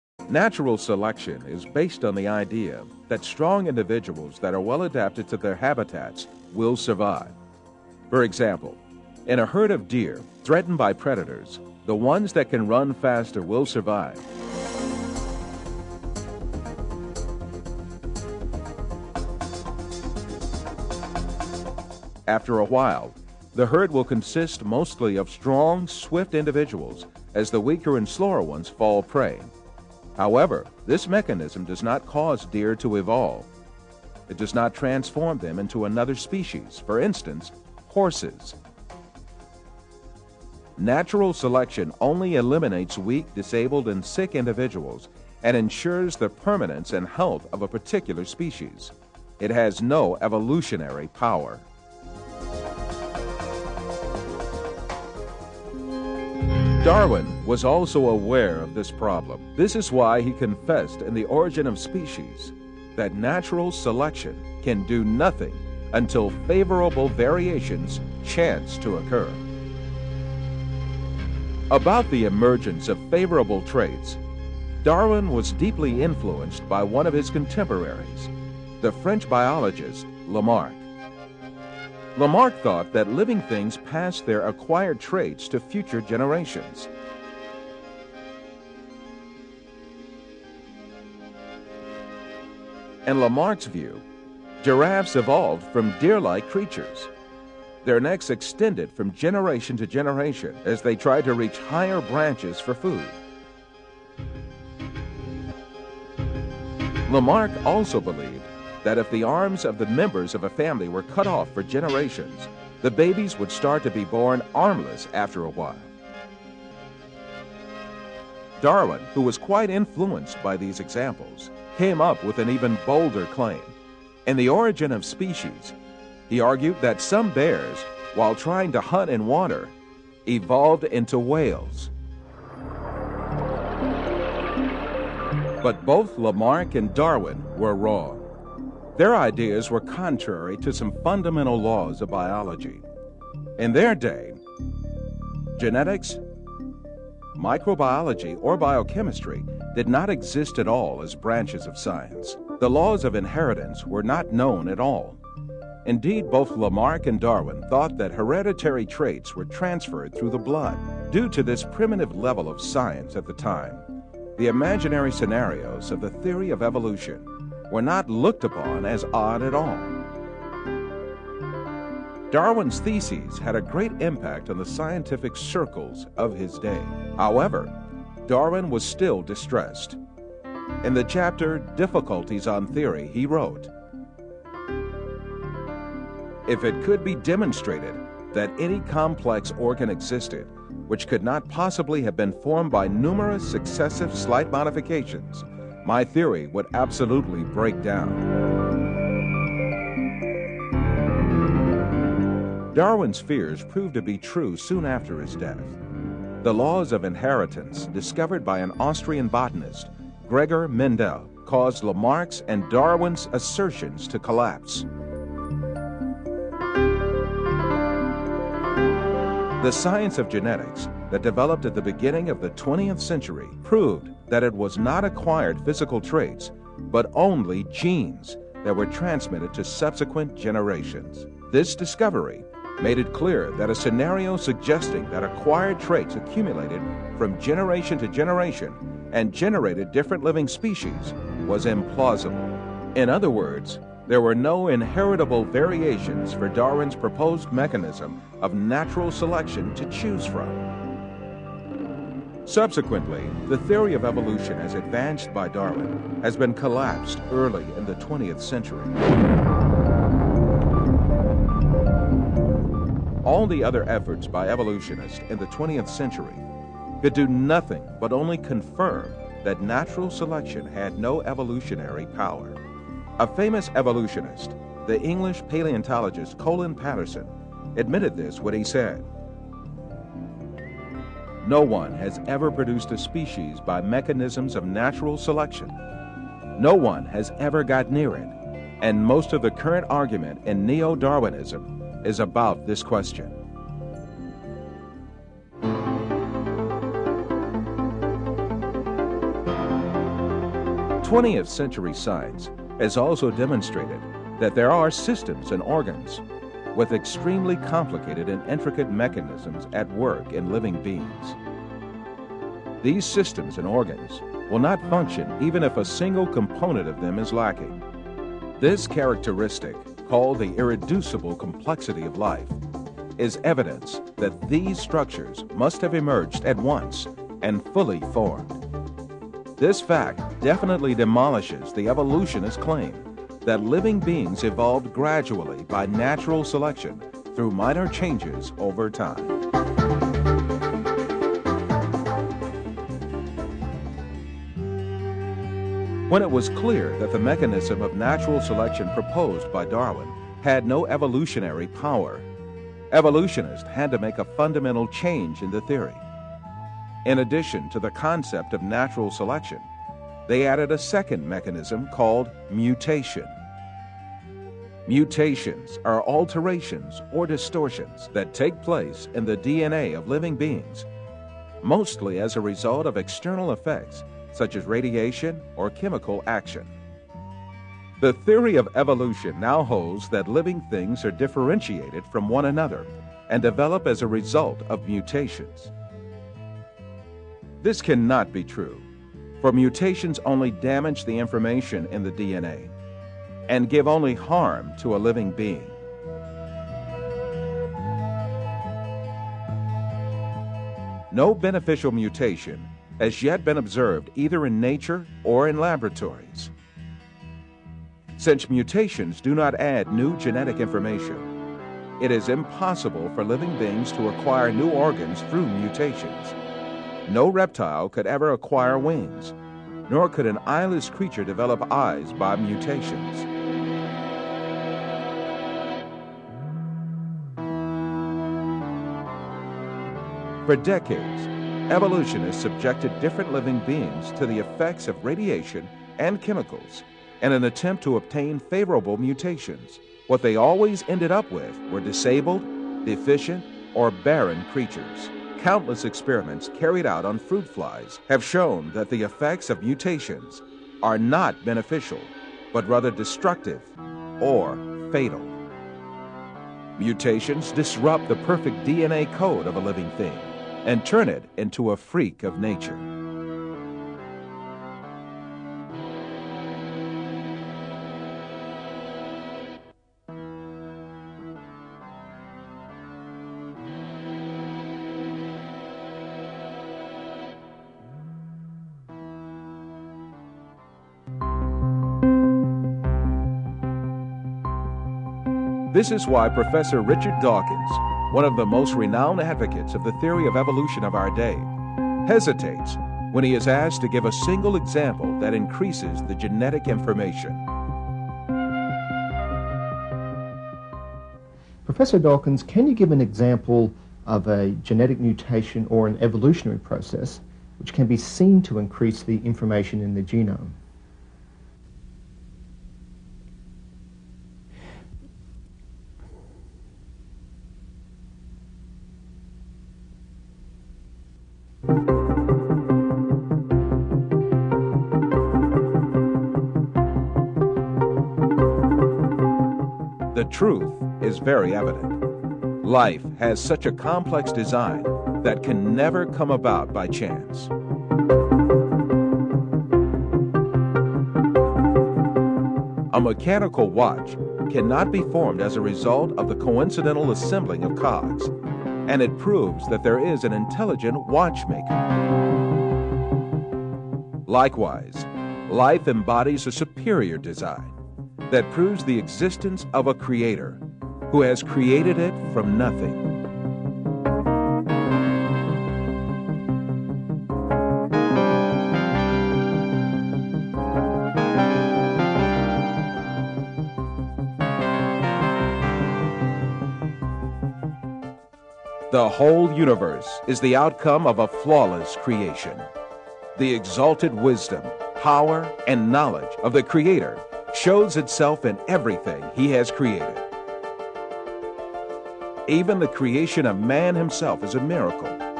Collapse Of Evolution [Theory Of Evolution Debunked] Evolution vs Creationism Part 3/7
Collapse Of Evolution [Theory Of Evolution Debunked] Part 3/7 . Charles Darwin's Theory Of Evolution Flawed by Modern Science and Technology. Evolution vs Creationism